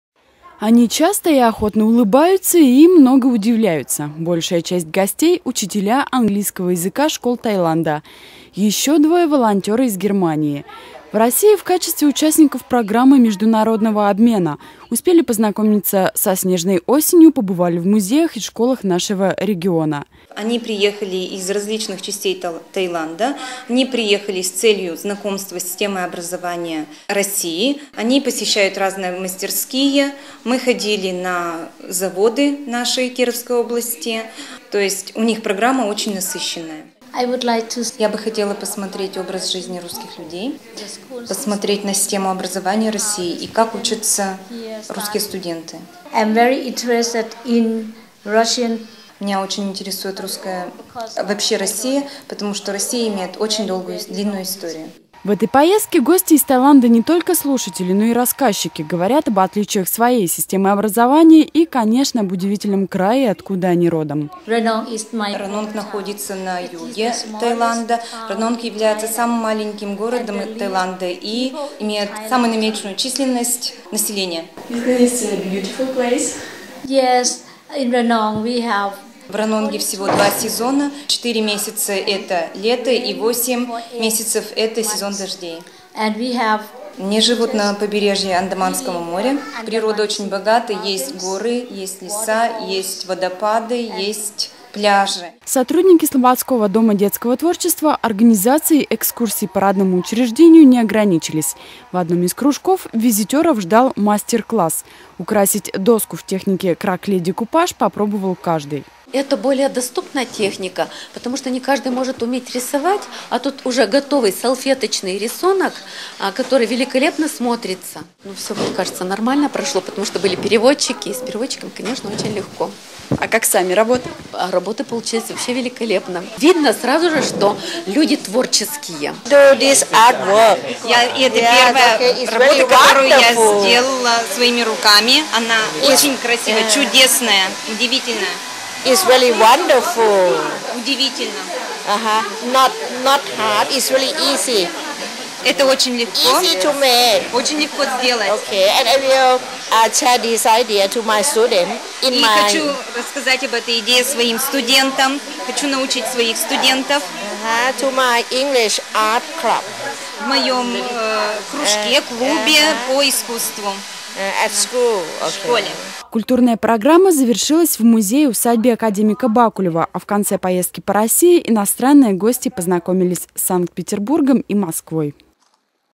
ДЕЛЕГАЦИЯ ИЗ ТАЙЛАНДА И ГЕРМАНИИ В СЛОБОДСКОМ
В минувшие выходные Слободской встречал делегацию из Тайланда и Германии. Гости знакомились с особенностями российского образования, побывали в музеях, а заодно попробовали свои силы в искусстве.